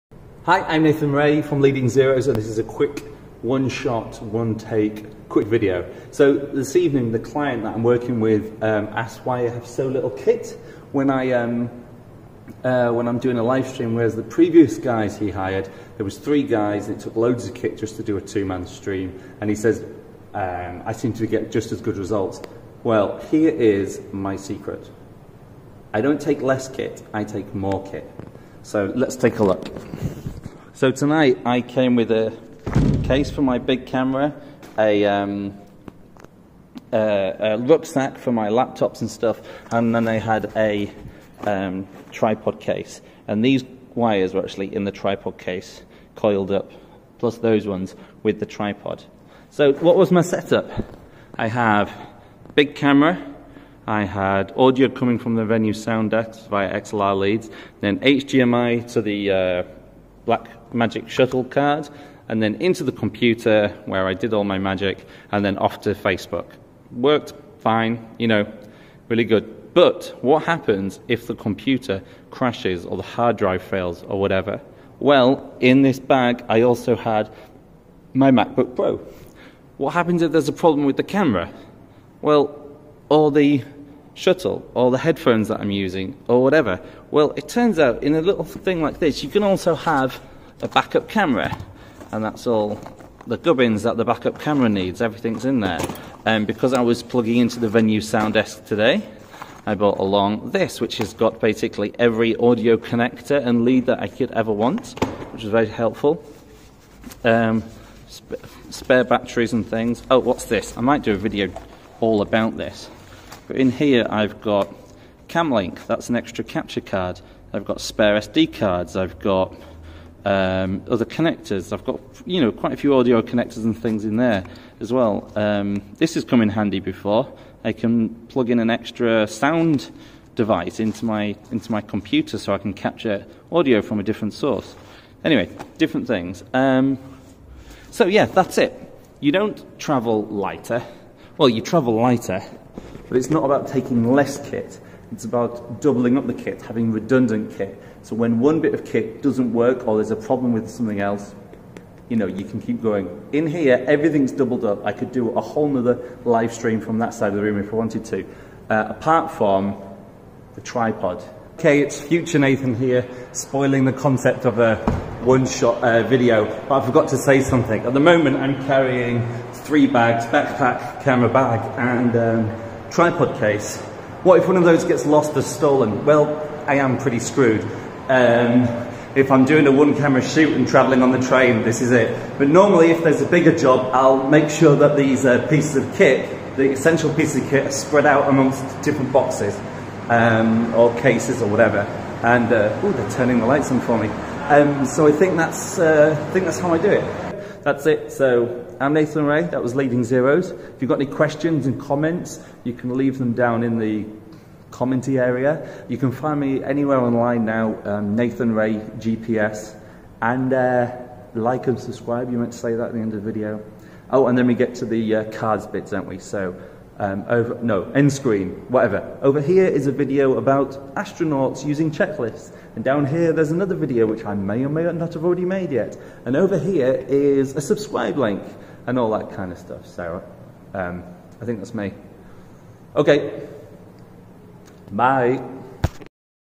How To Pack Light for Live Streaming - Leading Zeros Short
Why I actually take way too much gear when hired to live stream.